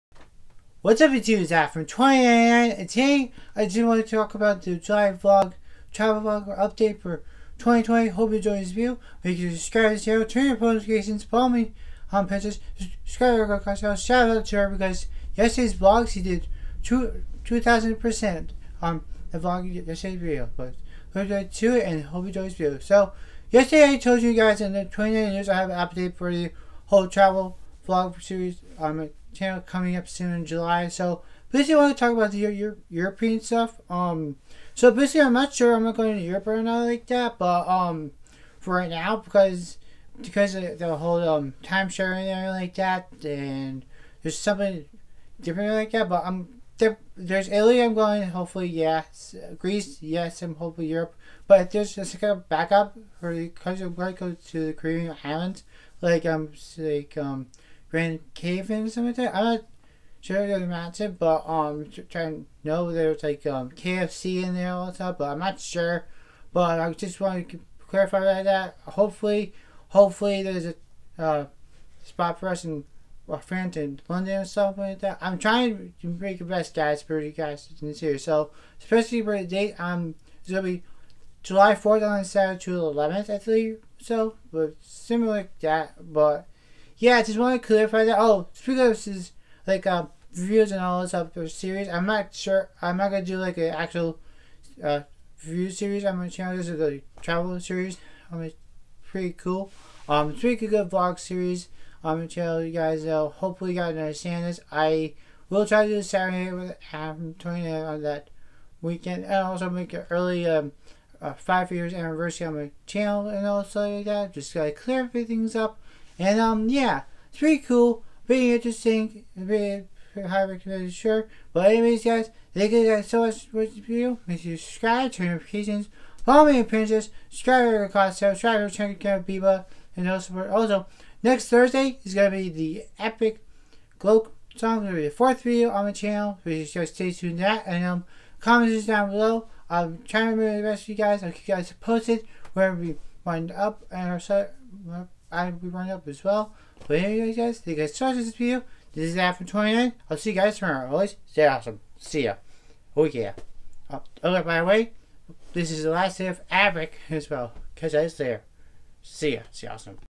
Travel vlog update series for July 2020 🇬🇧🌴
My awesome Europe tour 2020
July2020 dates will be coming soon updates ?Come show up its free
Paris,France
London, England
Brighton, England
Roman, Italy
Berlin , Germany
Bern, Switzerland
Brussels , Belgium